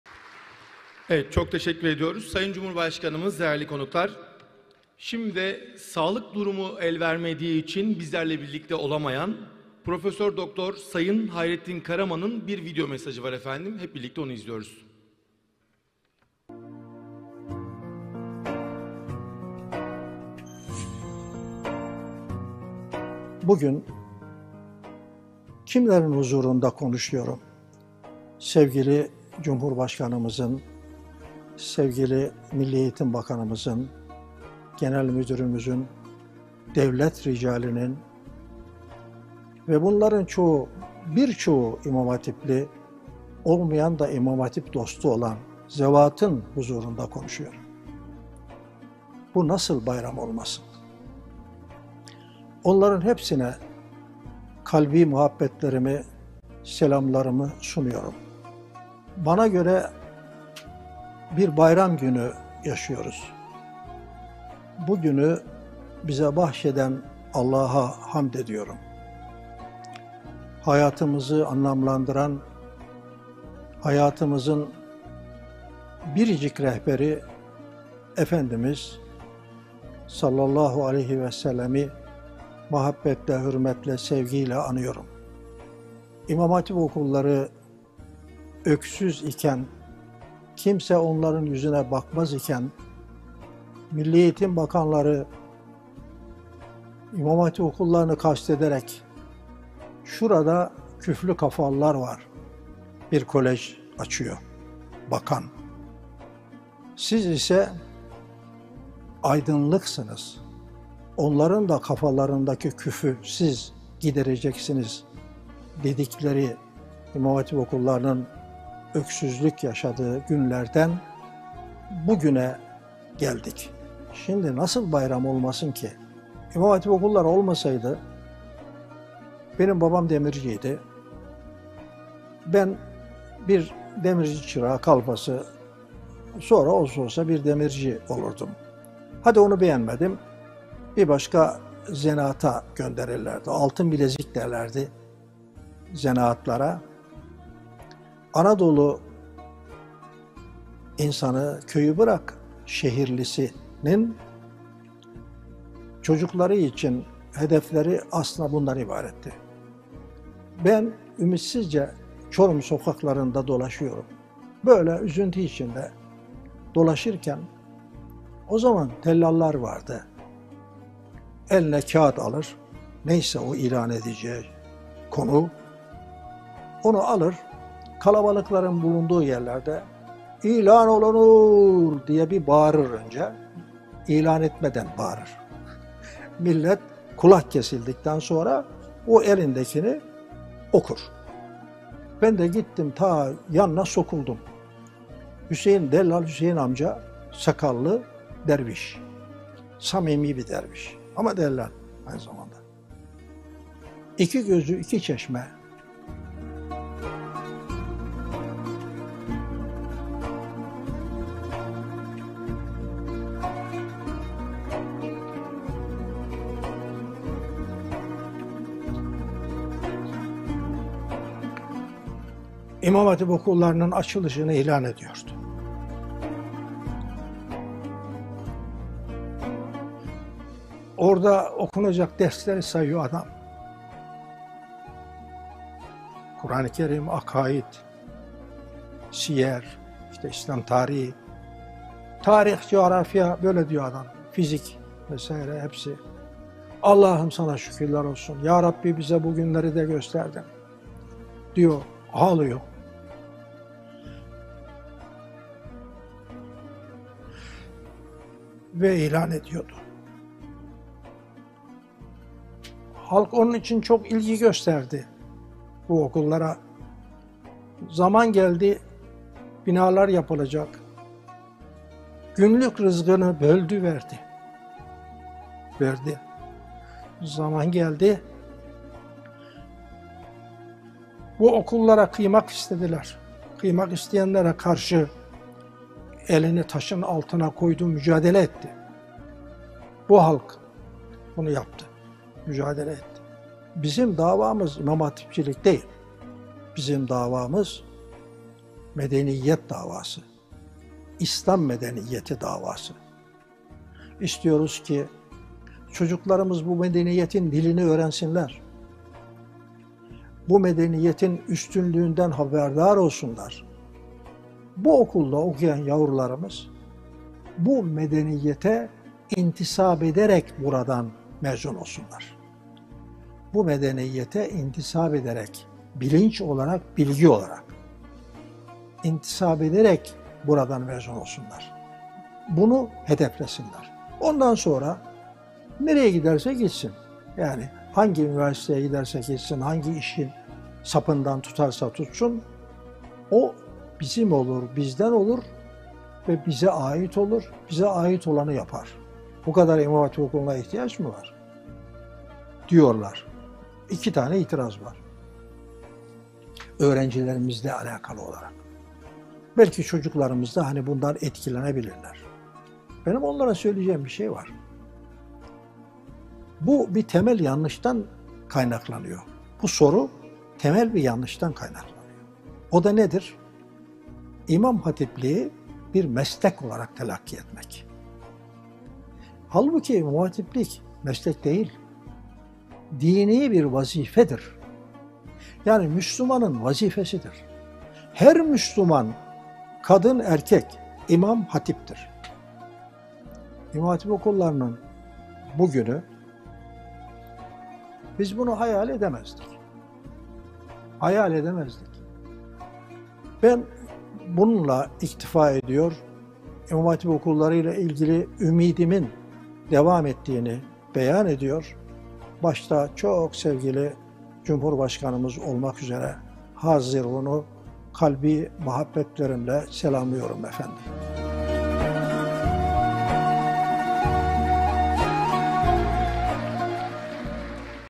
Prof. Dr. Hayrettin KARAMAN'ın, İmam Hatipler Temalı Konuşması
70.  Yılında İmam Hatip Okulları ve Türkiye'de Din Eğitimi Uluslararası Sempozyumu - Prof. Dr. Hayrettin KARAMAN